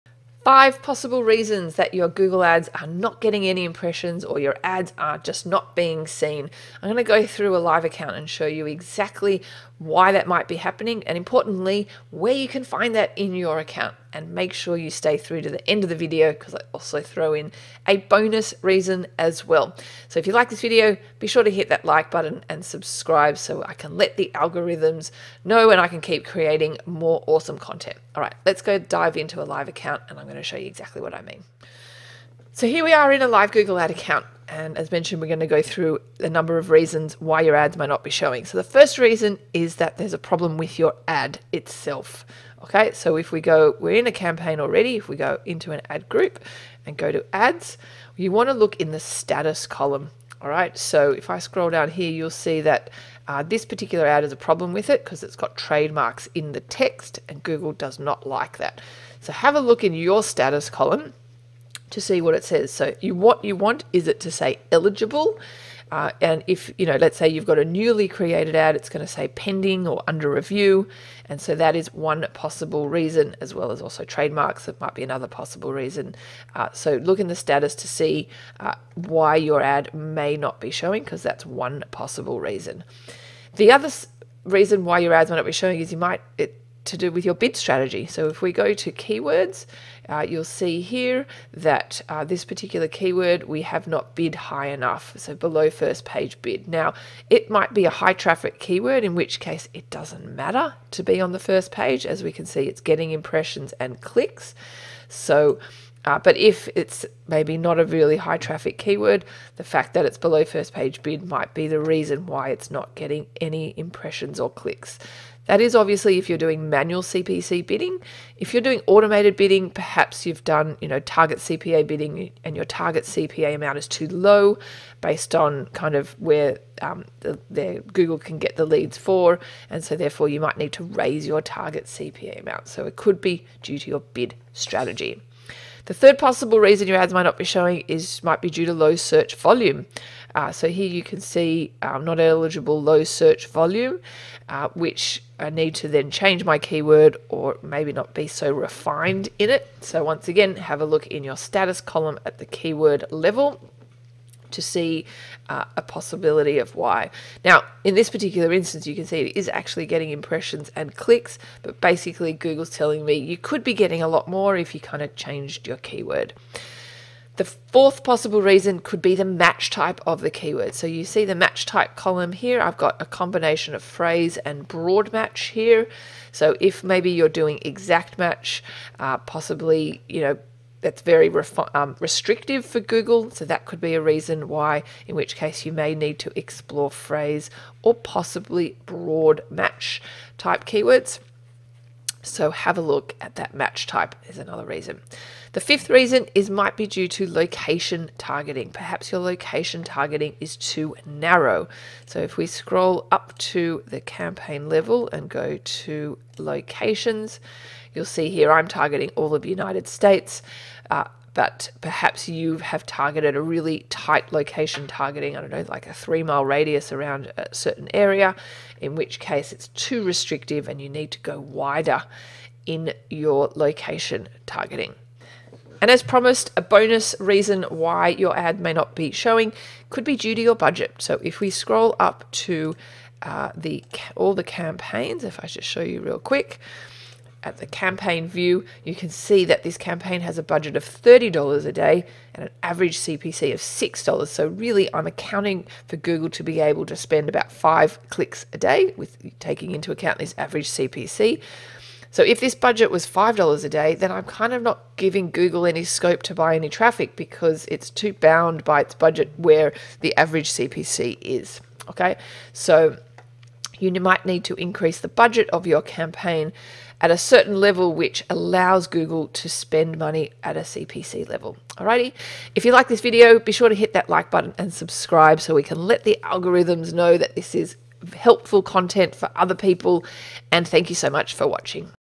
Google Ads Not Getting Any Impressions? Here Are The 5 Possible Reasons Why
Google Ads Not Getting Any Impressions? Here Are The 5 Possible Reasons Why - In this video I'll be showing you the possible reasons why your ads are not serving any impressions even though you already have created a campaign or ads and already approved.